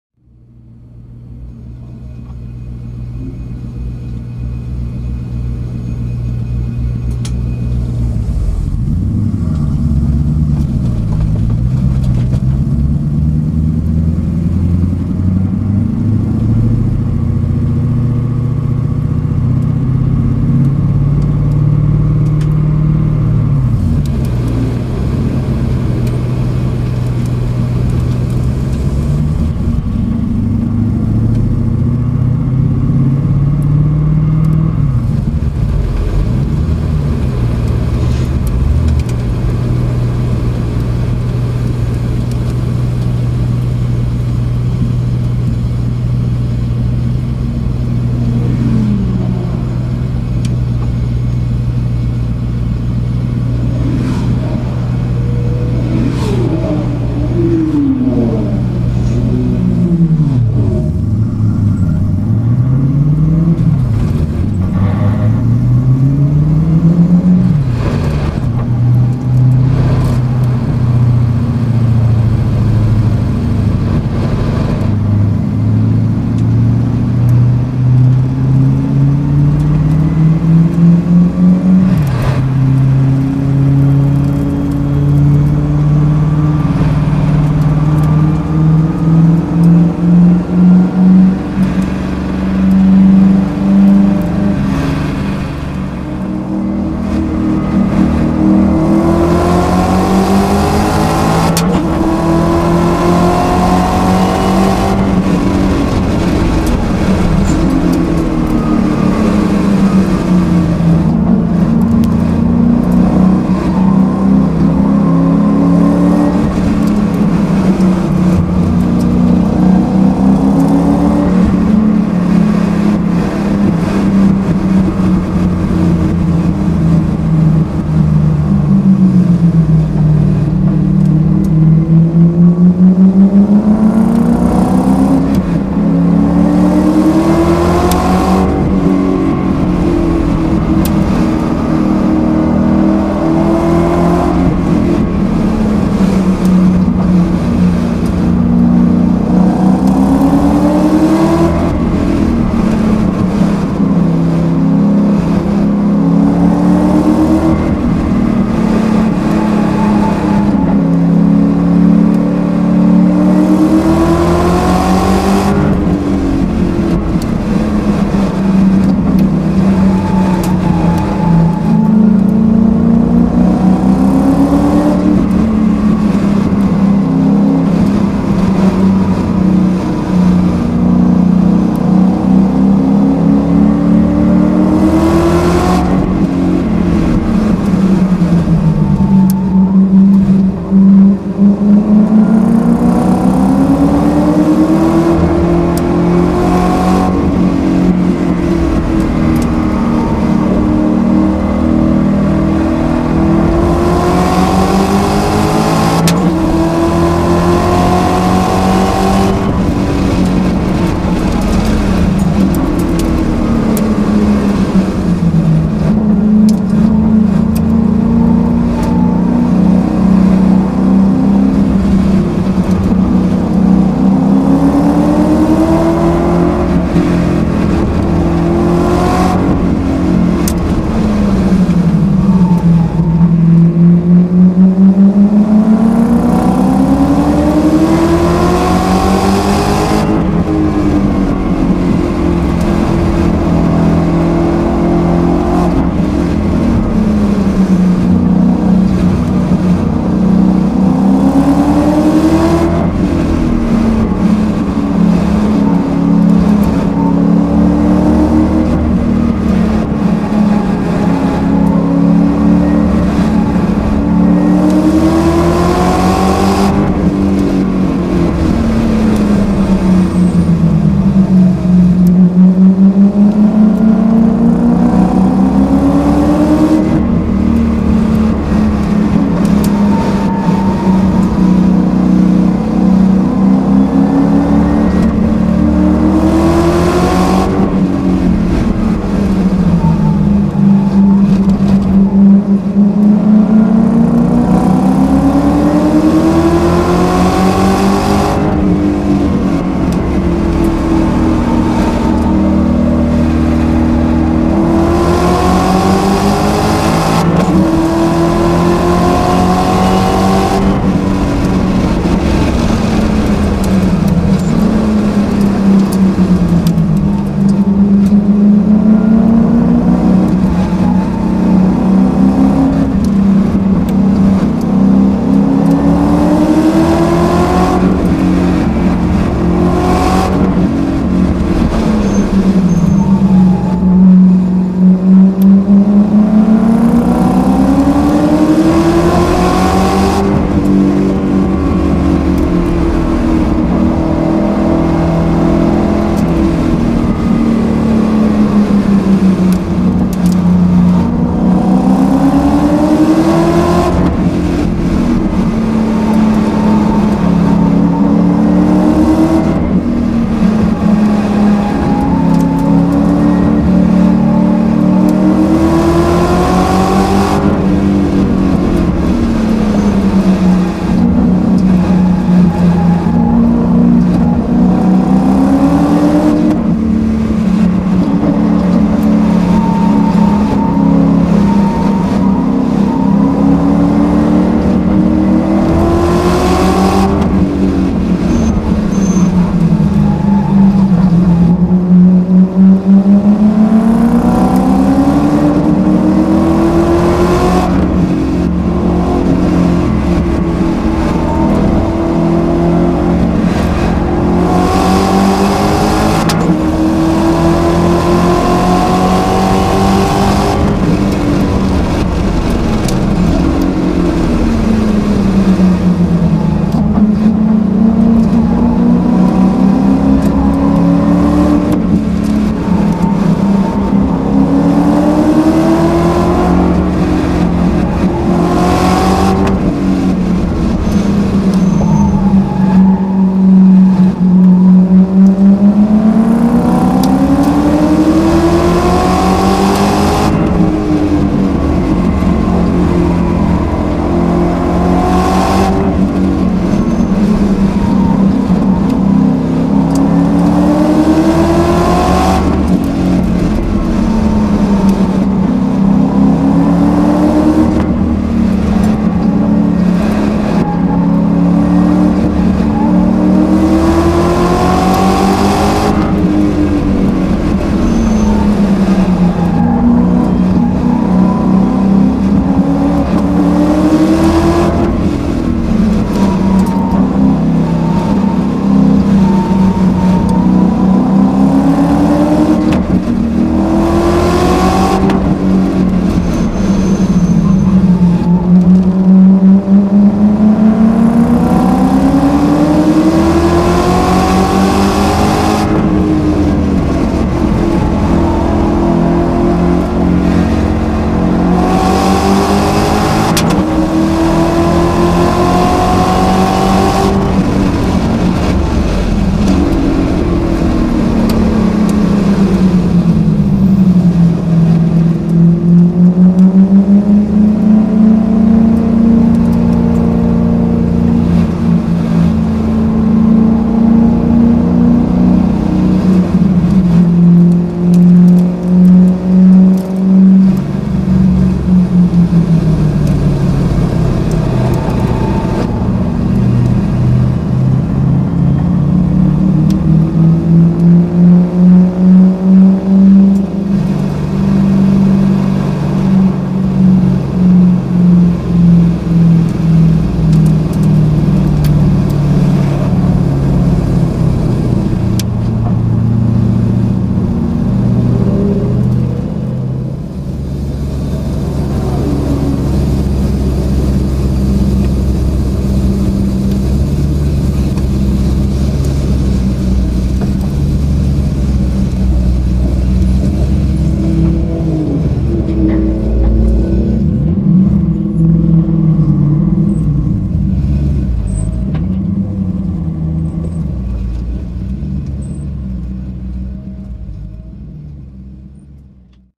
RS8 GRC May 2019 no1
Audi S8/RS8

Chasing a friend in his supercharged BMW M3 and have  another friend in his home built pick-up race car behind me in this movie.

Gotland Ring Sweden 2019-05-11
First laps of the day and haven't driven my car for over 1.5 year!

Now with MaxxEcu installed and tuned myself so only low boost to test everything this weekend.
So all gauges is from my MaxxEcu LOG file.

Also driving on my 5 year old tyres so it was getting more and more slippery thru the day.